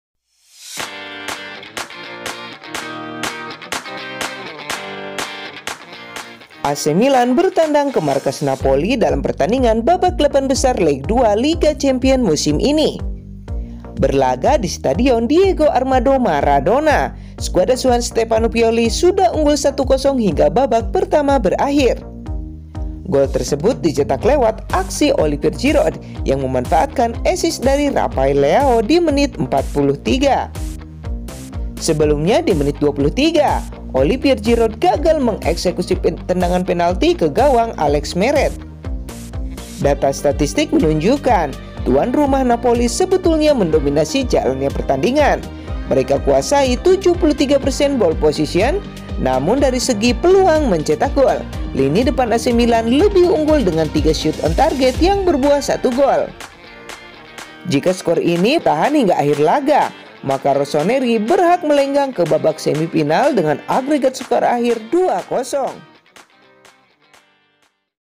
AC Milan vs Napoli Tadi Malam | Hasil Liga Champion Tadi Malam | Hasil Bola Tadi Malam
AC Milan vs Napoli tadi malam dalam hasil liga champion tadi malam atau hasil bola tadi malam di babak 8 besar leg 2 liga Champion 2023.
Hasil AC Milan tadi malam berakhir dengan skor 1-0 hingga babak pertama berakhir,gol AC Milan dicetak oleh olivier Giroud 43',AC Milan lolos semifinal liga Champions 2023